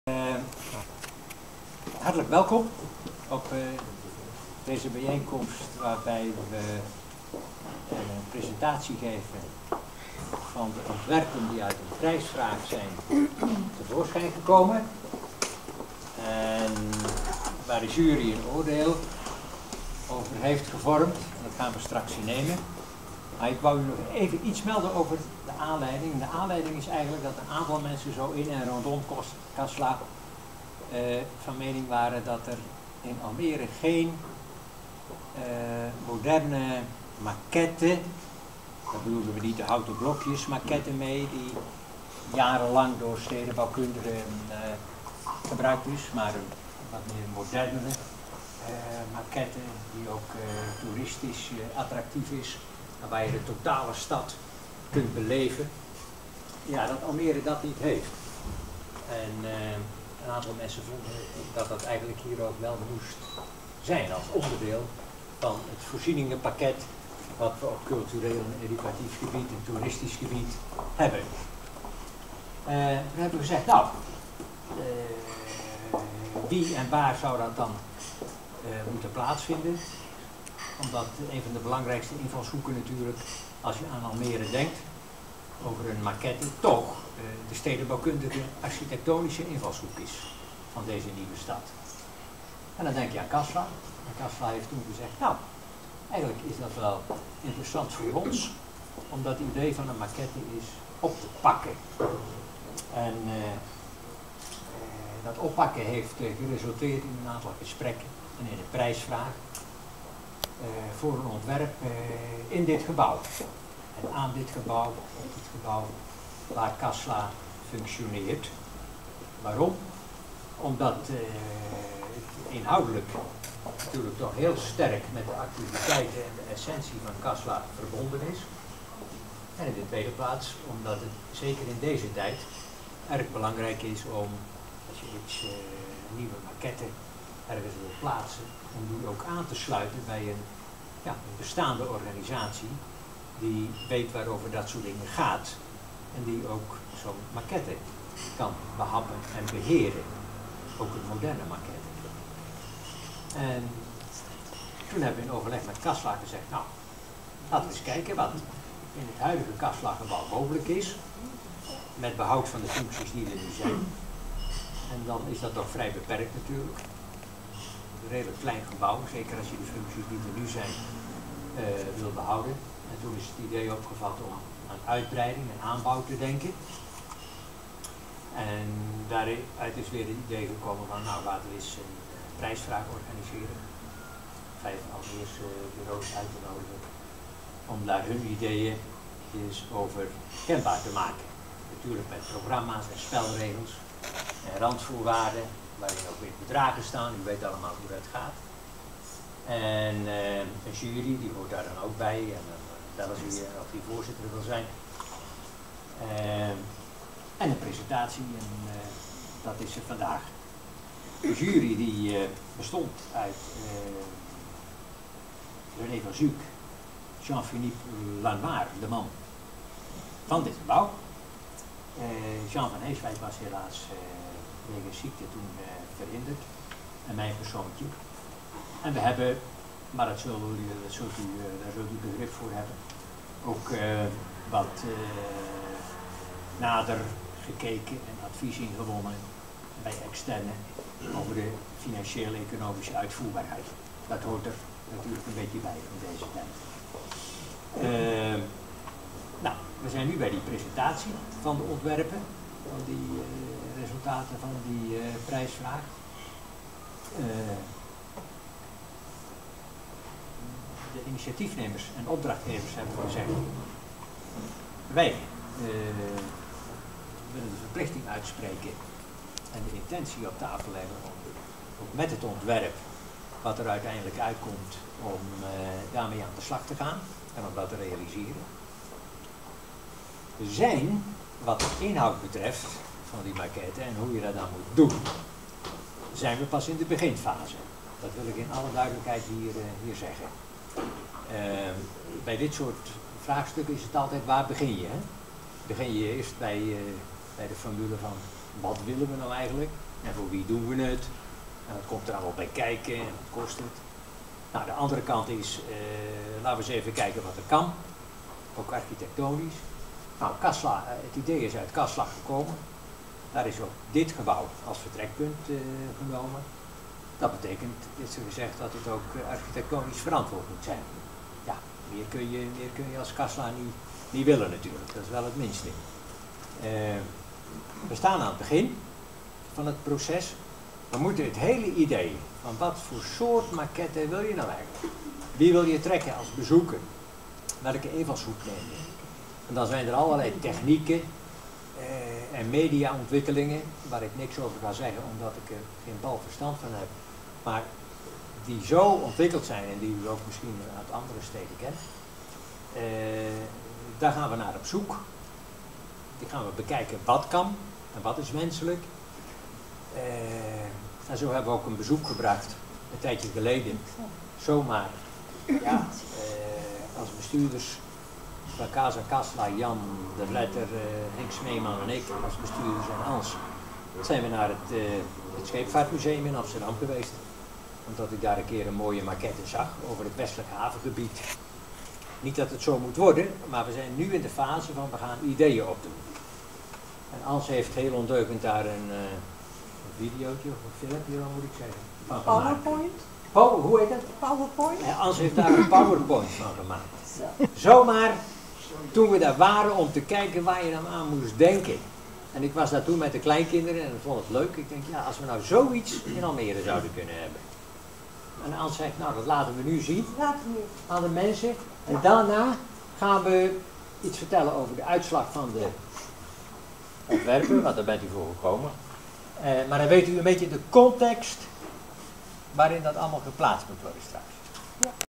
Kunst-enzo 2012 - Panorama Almere
20120419 Presentatie Panorama Almere in Casa Casla door Cees de Cloe (jurylid en bestuurslid Casla) en Ans van Berkum (direkteur Casla).
Architektuurcentrum Casla wilde graag een Stadsmaquette voor Almere; heeft een ontwerpwedstrijd uitgeschreven voor architekten; circa 250 inzendingen, waaruit er 5 werden geselekteerd. Op deze middag werd bekend gemaakt wie de winnaar van deze 5 zou worden (dat werden er uiteindelijk 2 omdat die het meest voldeden aan de opdracht, de randvoorwaarden, het eisenpakket, enz. Met deze twee komt binnenkort een verder gesprek nav hun ontwerp) Naar financiering wordt nog gezocht...
In de JURY zaten: Cees de Cloe (oud-burgemeester van Almere), Jean-Philippe Lanoire (de architekt van het bestaande gebouw CASA CASLa), René van Zuuk (Almeerse architekt) en Jeanne van Heeswijk. (meedenkers voor dit initiatief war oa Henk Smeeman (oud-wethouder Almere en Jan de Vletter (oud-direkteur woningb.ver.GSA (Groene Stad Almere) en oud-raadslid Almere)